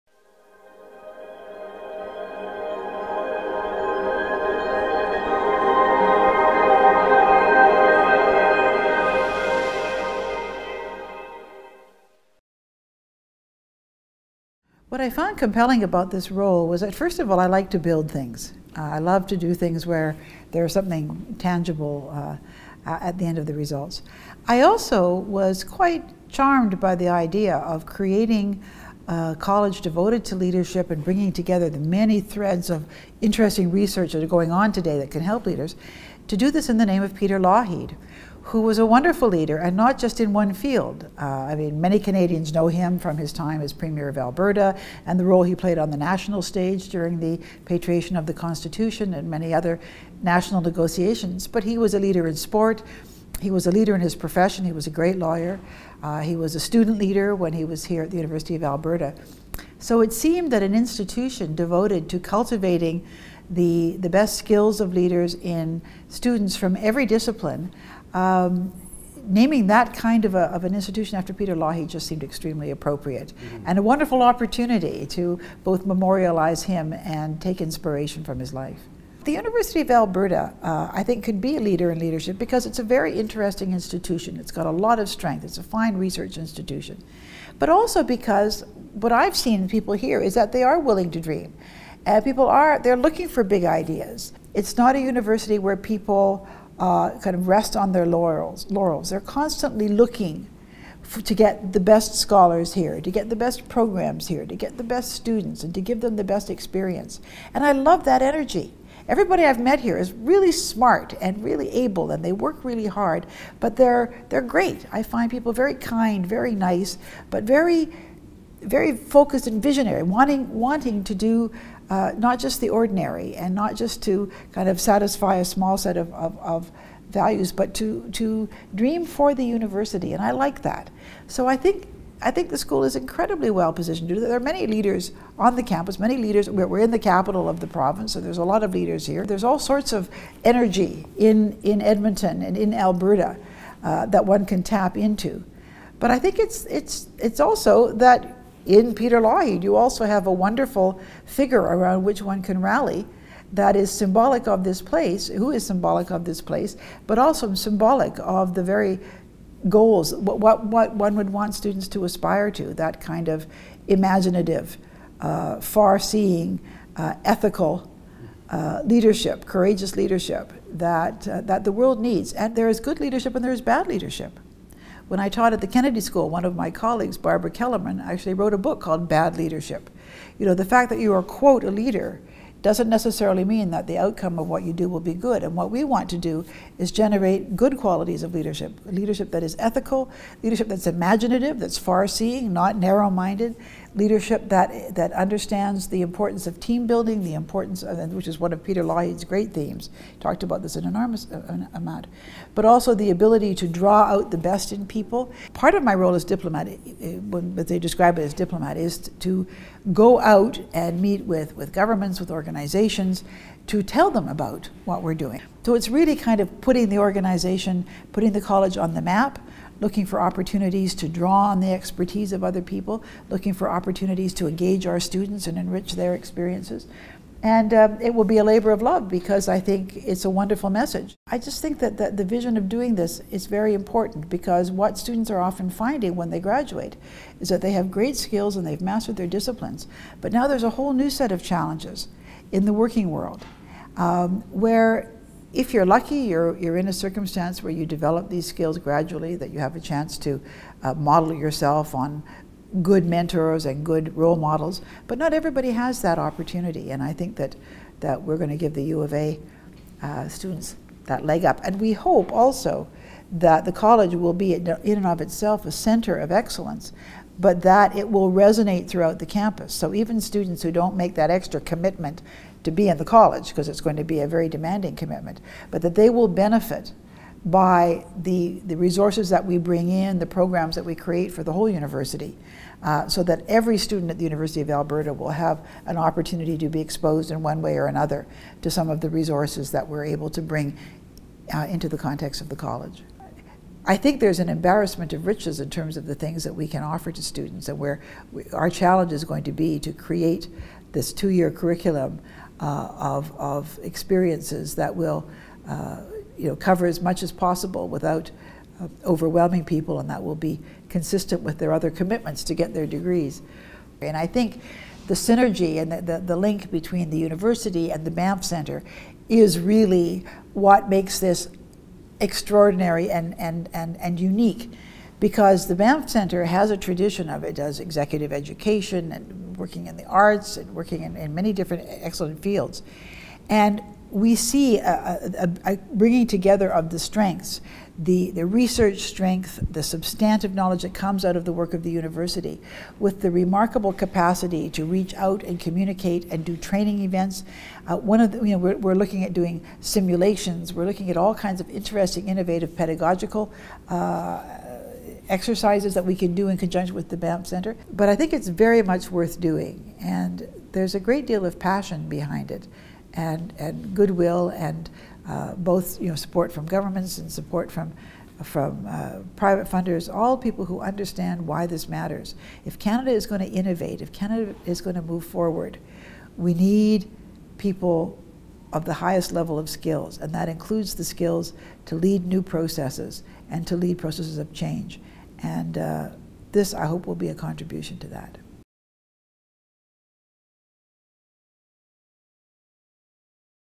Former PM Kim Campbell to lead Peter Lougheed Leadership College
The first and only Canadian woman to lead the nation as prime minister will oversee the creation of the Peter Lougheed Leadership College at the University of Alberta.

The Right Hon. Kim Campbell, Canada's 19th prime minister, has been appointed founding principal of the college, one of the two main elements of the Peter Lougheed Leadership Initiative, a collaboration with The Banff Centre aimed at creating one of the world's pre-eminent leadership development programs.